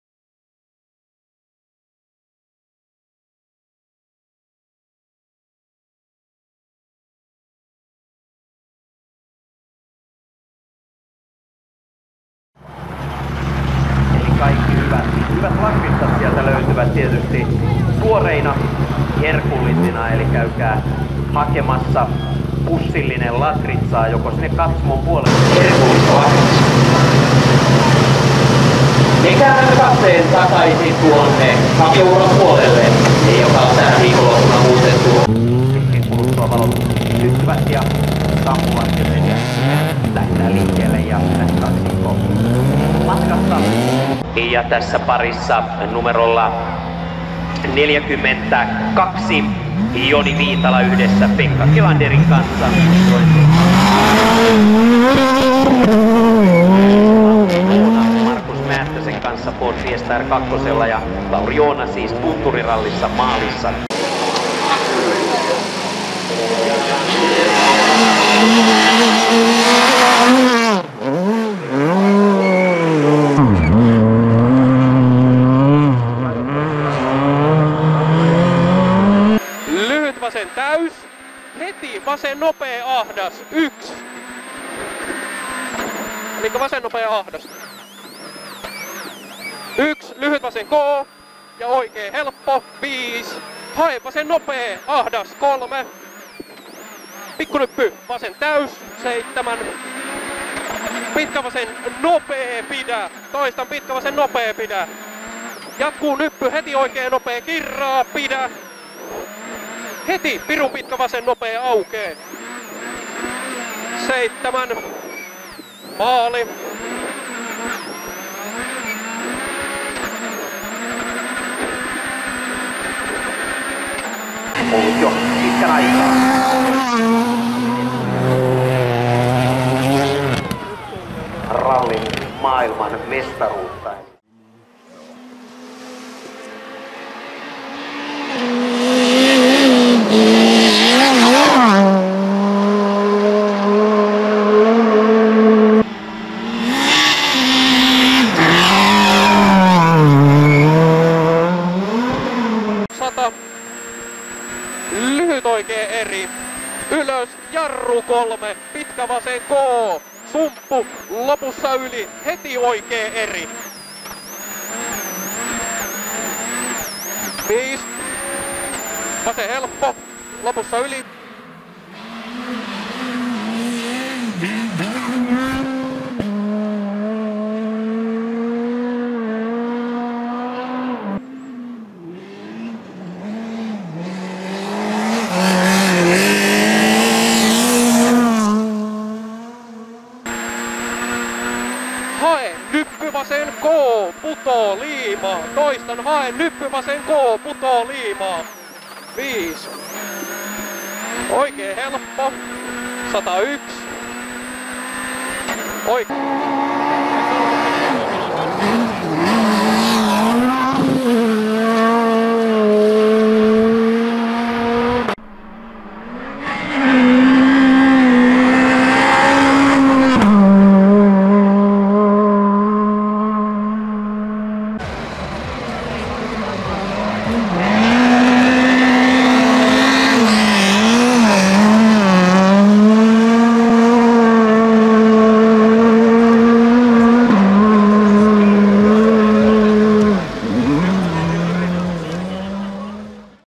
Viitala Racing // SM-Vaakuna Rally 2018
Tässä Viitala Racingin kisakooste Mikkelin Sm-osakilpailusta.
Joni Viitalan ja Pekka Kelanderin hyvin alkanut kisa päättyi ennen aikojaan nelospätkän jälkeen tekniseen vikaan.

Compilation of Viitala Racings Mikkeli SM-Rally.
Greatly started rally of Joni Viitala & Pekka Kelander came to a sudden end after ss4 because of technical failure